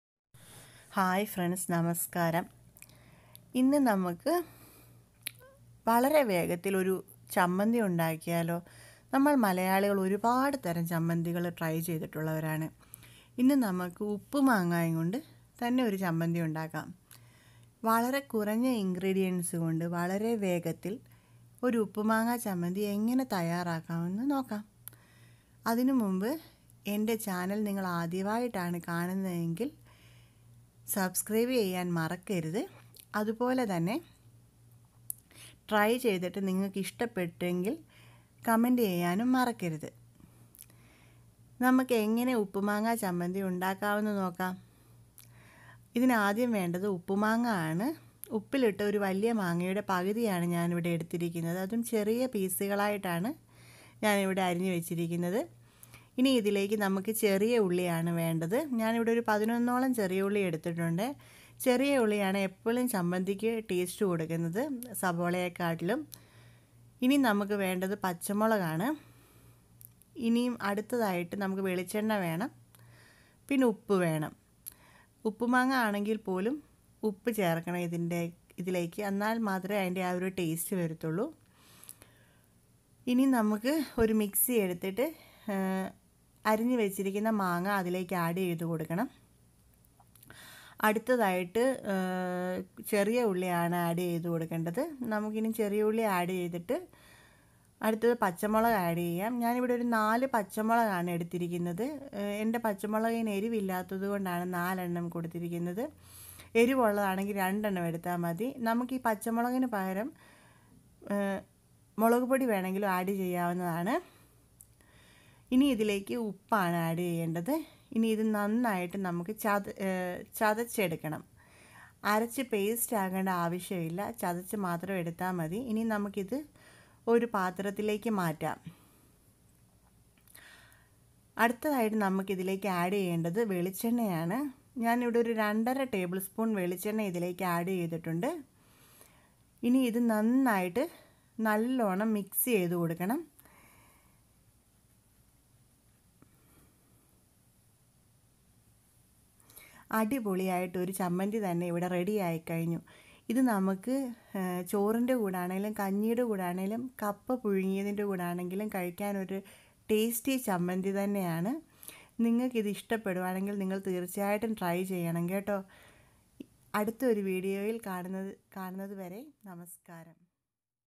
Easy and tasty southindian uppumanga chammadhi.
ഉപ്പിലിട്ട വിഭവങ്ങൾ നമ്മൾ മലയാളികൾക്ക് ഒഴിച്ച് കൂടാൻ പറ്റാത്ത കാര്യമാണ്... അപ്പോൾ അതുകൊണ്ട് ഒരു അടിപൊളി റെസിപി ആയാലോ.
INGREDIENTS
salt mango
shallot
coconut oil
greenchilly(chilly powder )
salt..